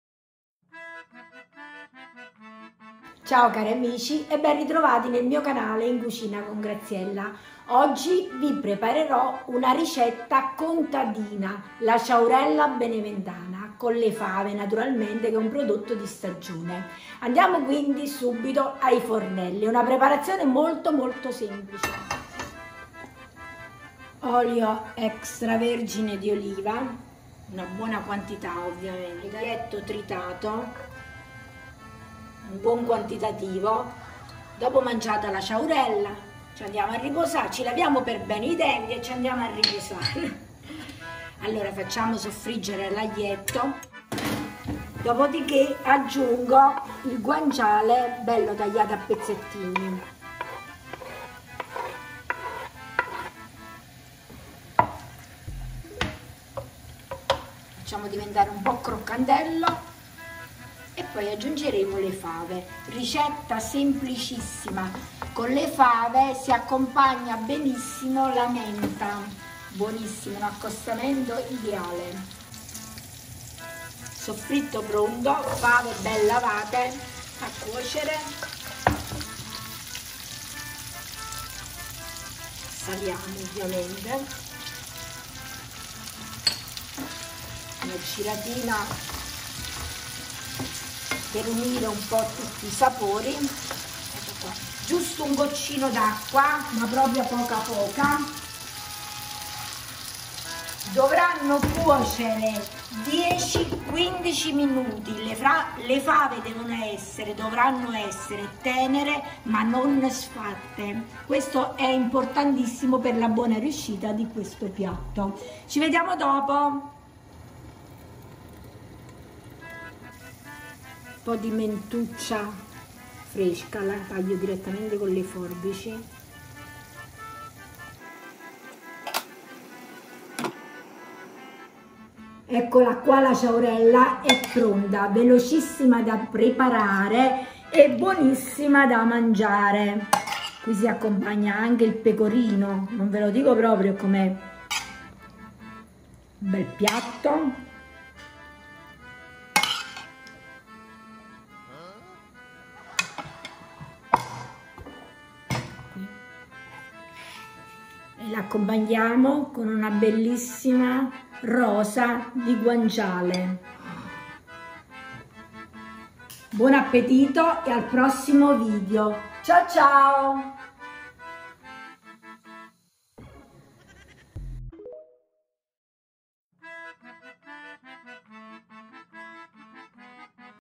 Fave fresche come si cucinano - Come si cucinano le fave fresche con la buccia - Ciaurella contadina
Prodotti Consigliati
Set Padelle